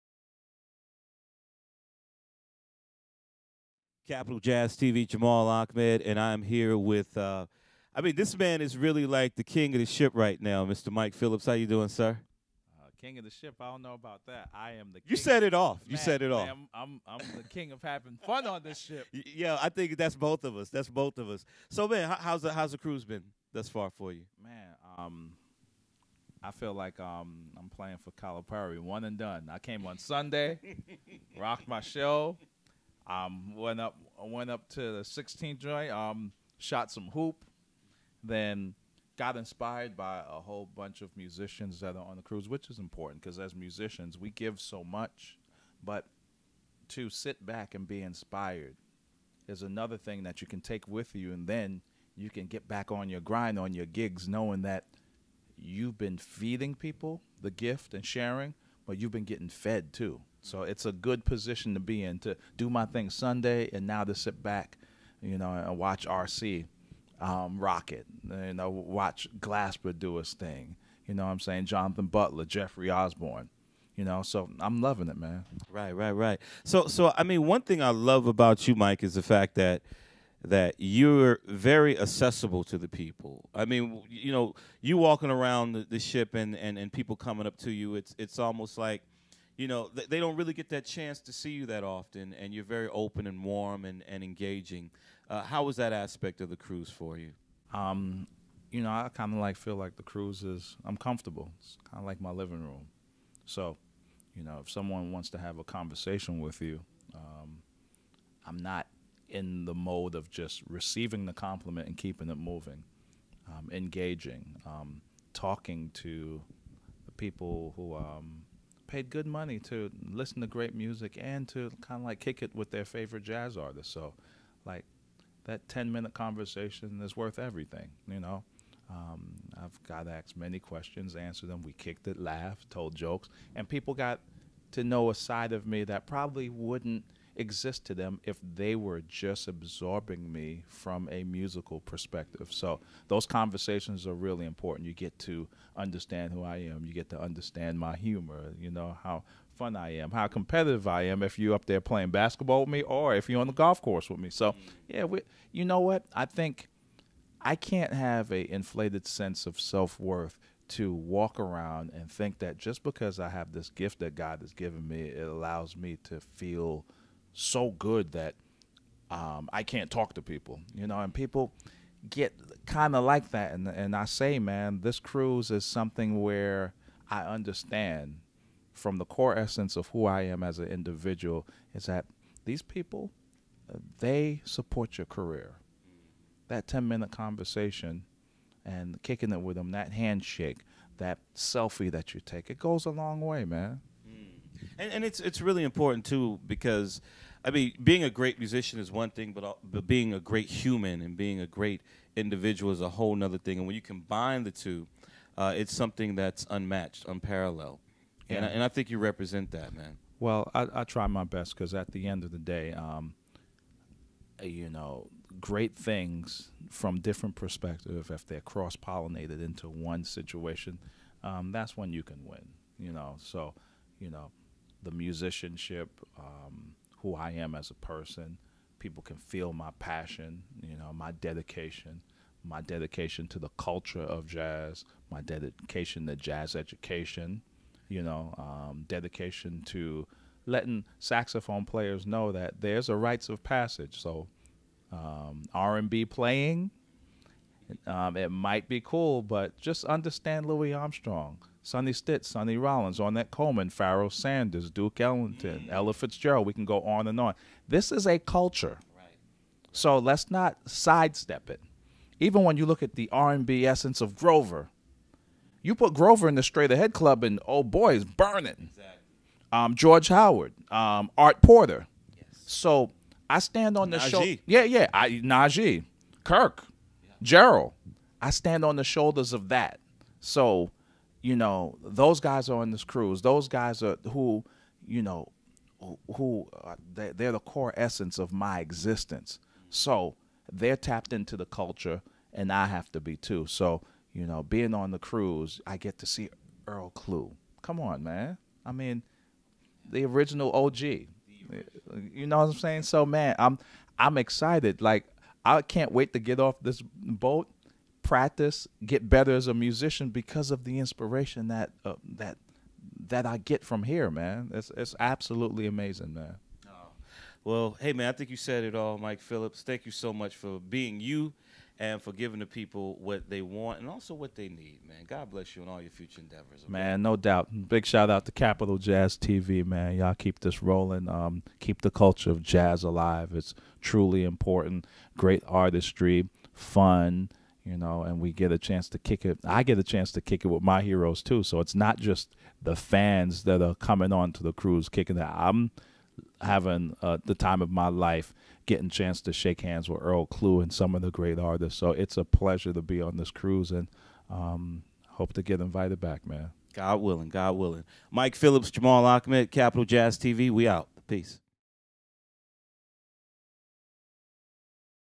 Mike Phillips Interview - 2017 SuperCruise
Interview with saxophonist MIKE PHILLIPS, hosted by Jamal Ahmad, during our 11th annual Capital Jazz SuperCruise, September 2017. See this and other behind-the-scenes video footage on our Capital Jazz TV channel